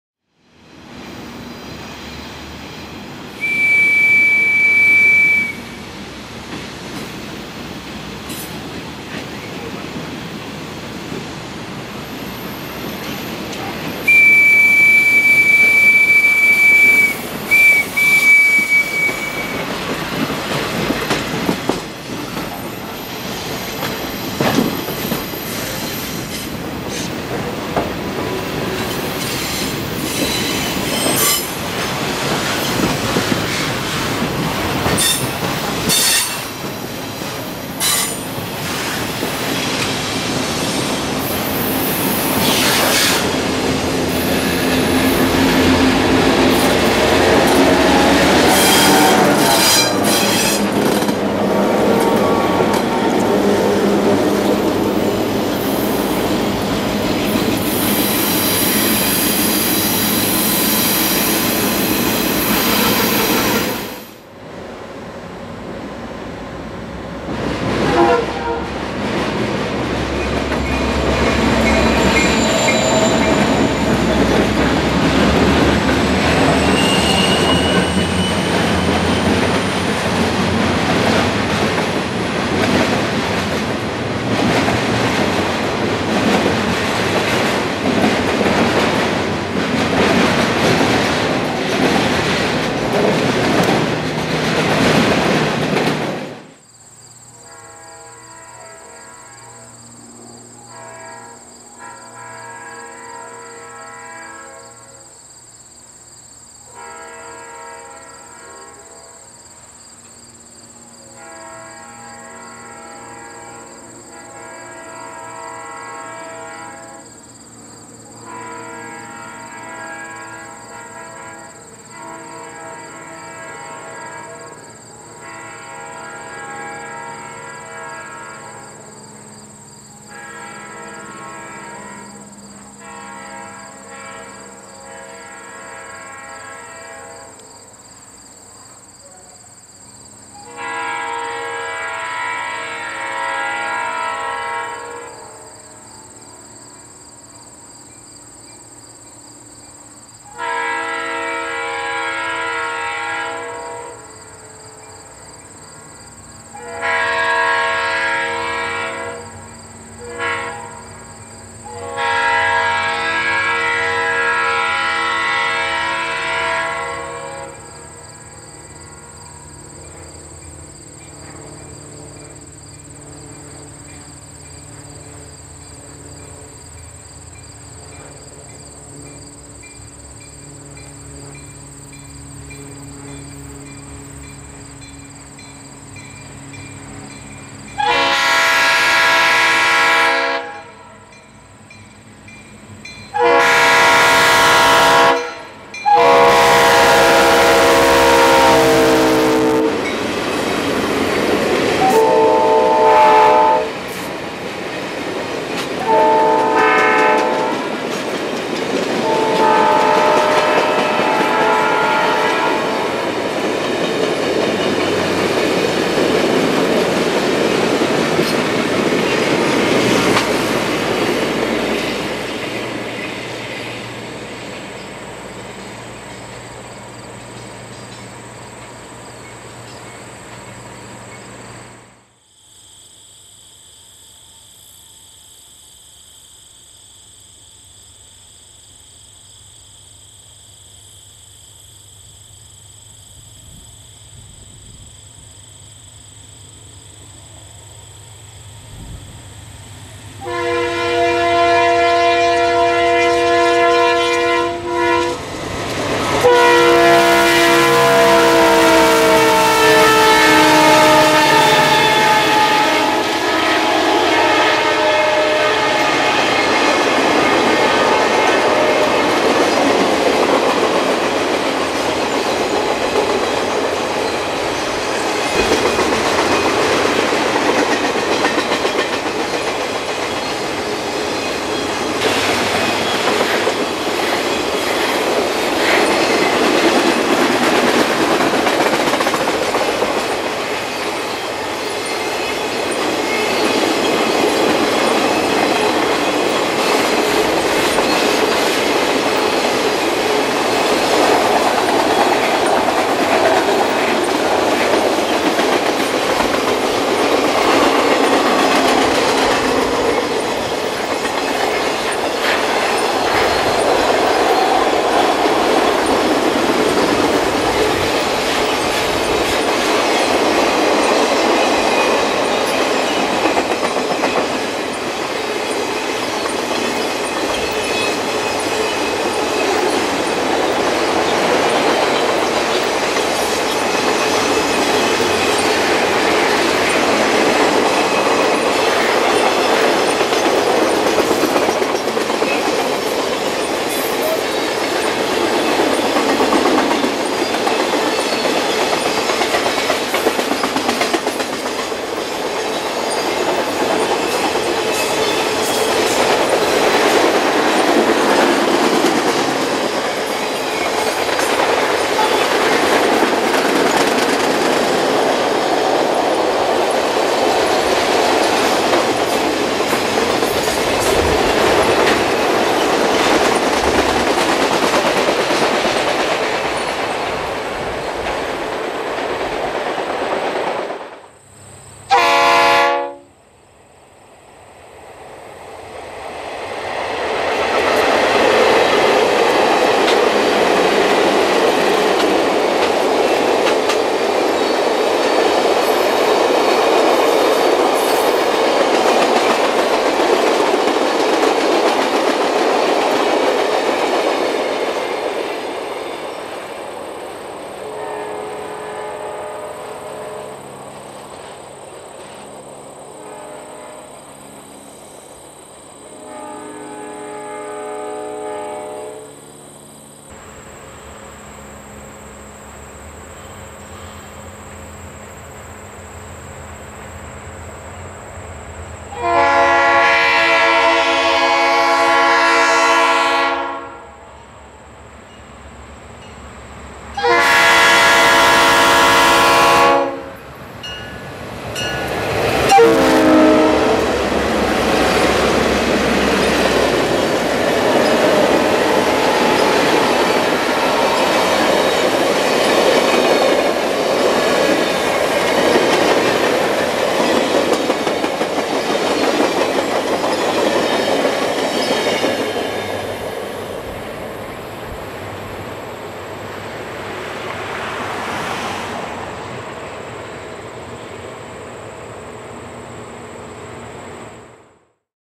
Mid-Ohio Conrail Potpourri
The first scene is a fiber optic work extra on the Delaware siding, followed by TV-4 at Marion, then an Operation Livesaver OCS at Jefferson St. in Marion. The OCS would run around its train at Galion, and return westbound (not shown in the video). Next is Mail-4 and the now returning OCS at "the prairie" about five miles east of Marion, then one last runby of the OCS at Campbell Rd. adjacent to the Whirlpool plant on the west side of Marion.